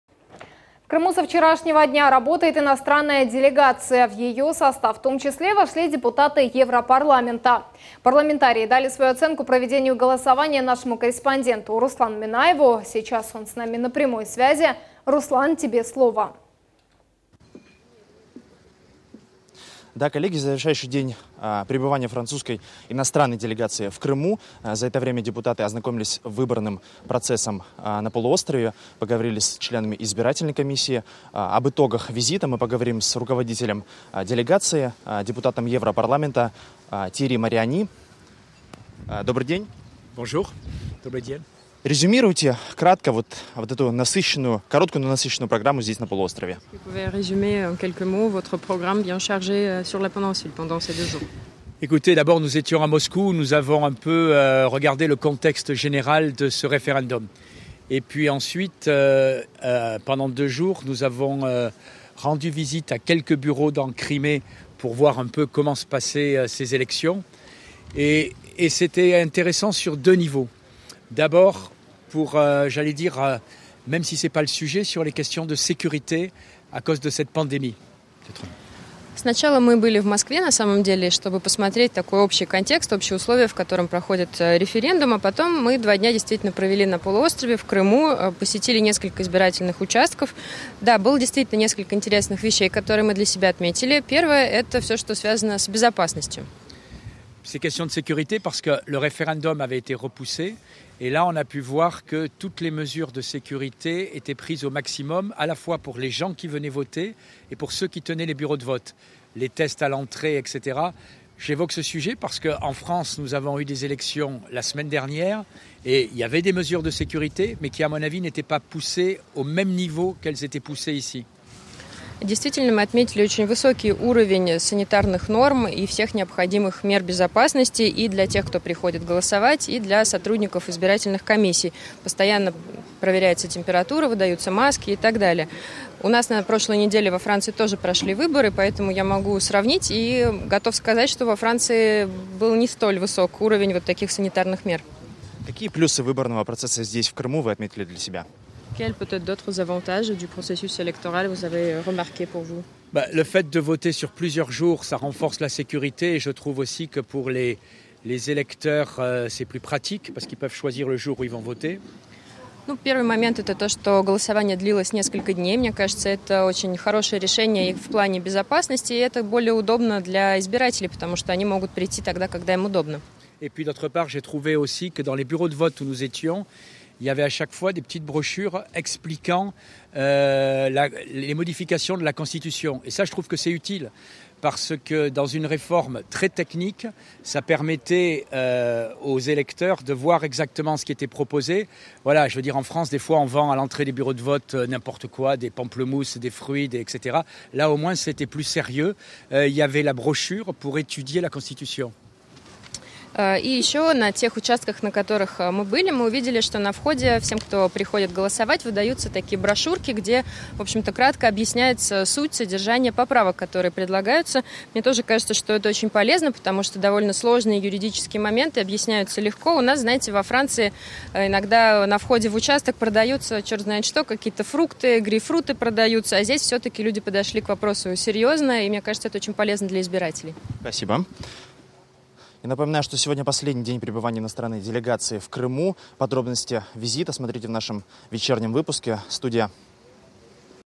Международное признание
В Крыму со вчерашнего дня работает иностранная делегация. В ее состав, в том числе, вошли депутаты Европарламента. Парламентарии дали свою оценку проведению голосования нашему корреспонденту Руслану Минаеву.